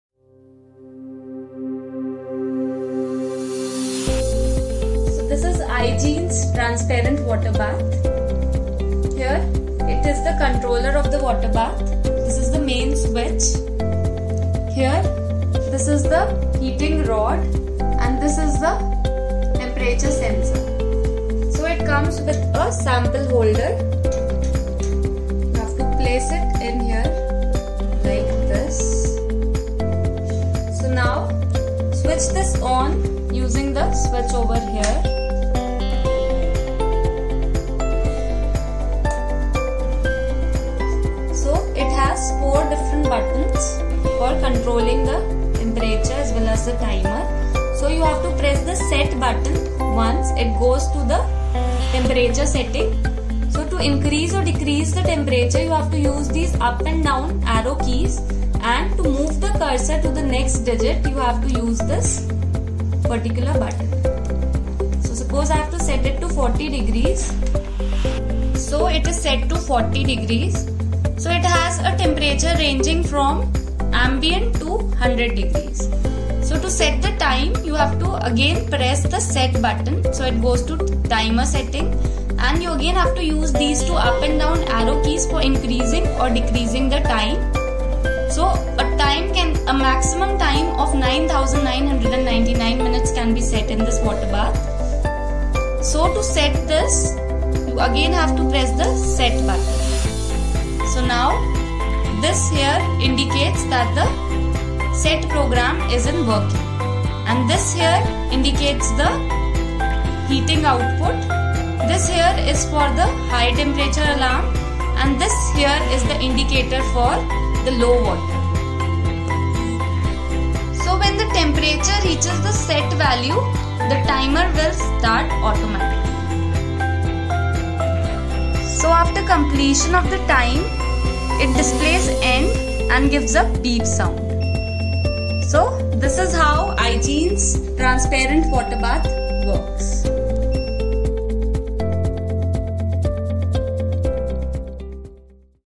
Transparent Water Bath | iGene Labserve
Transparent Water Bath's shell, interior and cover are all made of transparent engineering plastics which allows the clear vision of the samples. It comes with digital display and a soft touch button controller for setting various parameters.